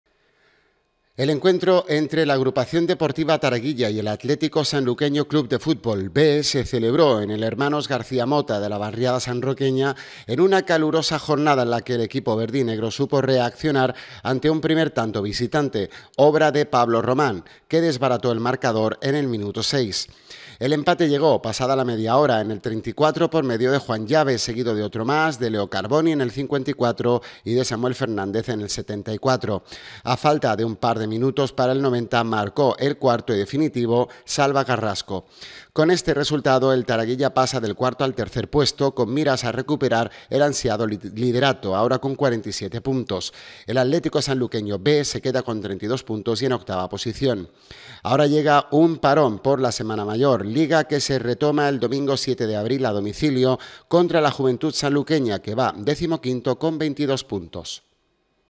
NOTICIA CRÓNICA AD TARAGUILLA 18 MARZO 2024
Taraguilla vence con rotundidad al filial del Sanluqueño (4-1)

Agrupación Deportiva Taraguilla recibió el domingo al Atlético Sanluqueño Club de Fútbol “B” en el encuentro correspondiente a la vigésimo-tercera jornada de Liga en Segunda Andaluza gaditana en la que los sanroqueños ganaron por un aplastante 4-1, metiéndose de nuevo entre los tres primeros puestos de la categoría.